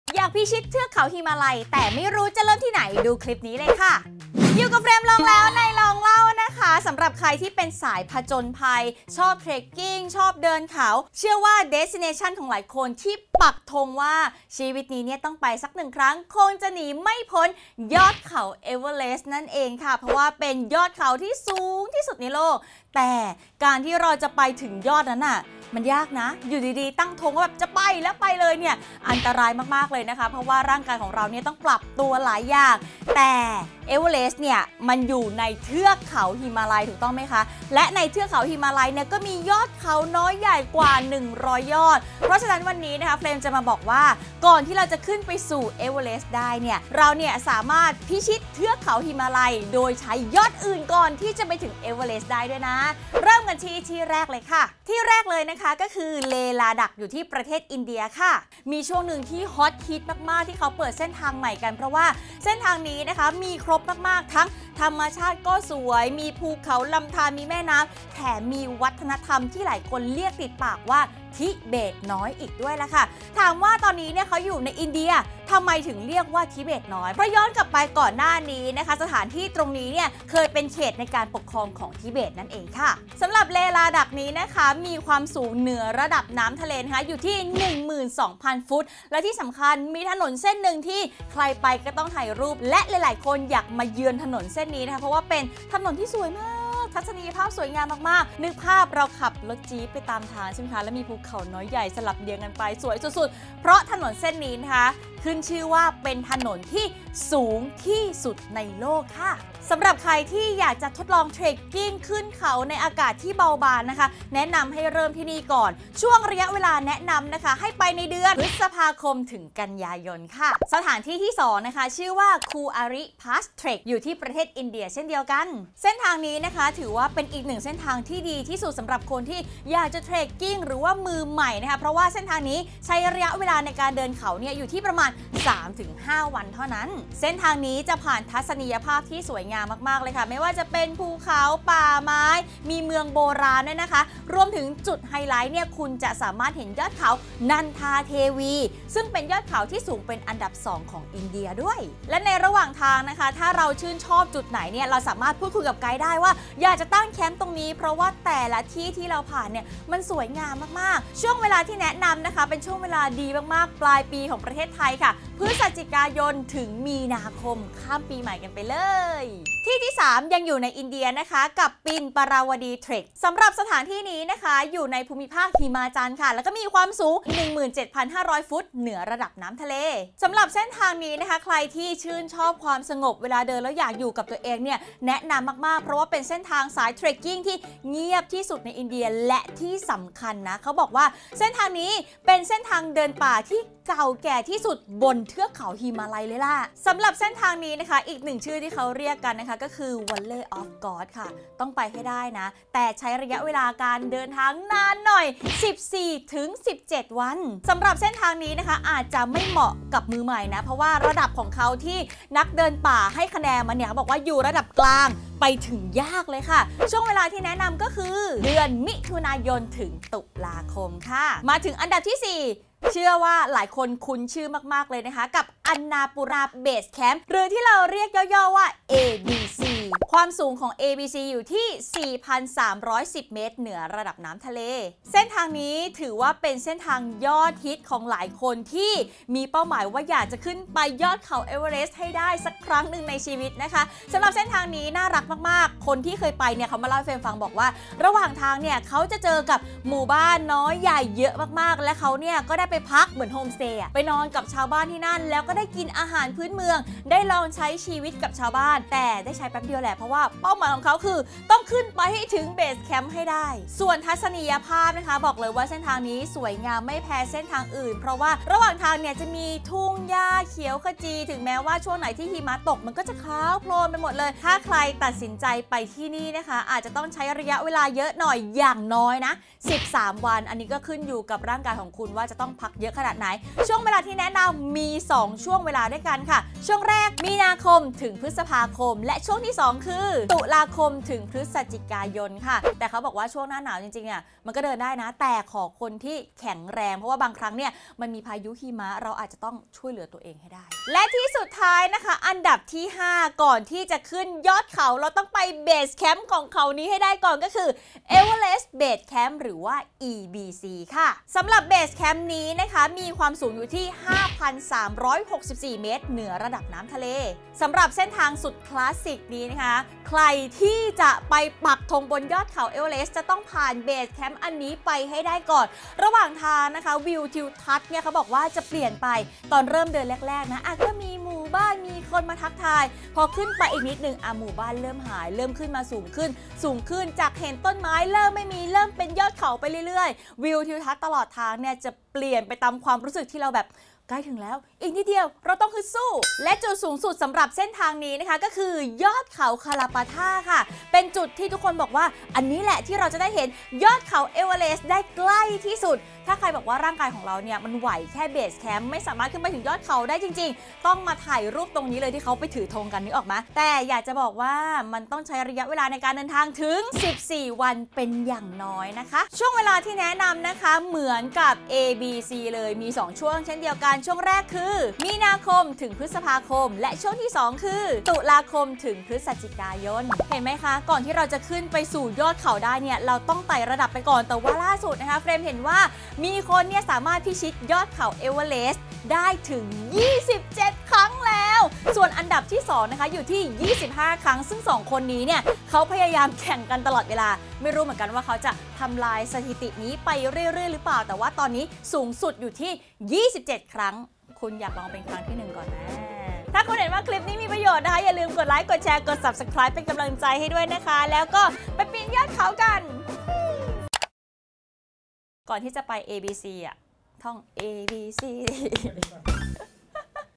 5 เส้นทาง เหยียบเทือกเขาหิมาลัย | ลองเล่า | EP.60
ก่อนจะขึ้นสู่ยอด Everest เราลองไปยอดเขาอื่นๆ ของเทือกเขาหิมาลัยดูไหม เรารวบรวมมาให้แล้ว 5 เส้นทางที่ได้เหยียบเทือกเขาหิมาลัย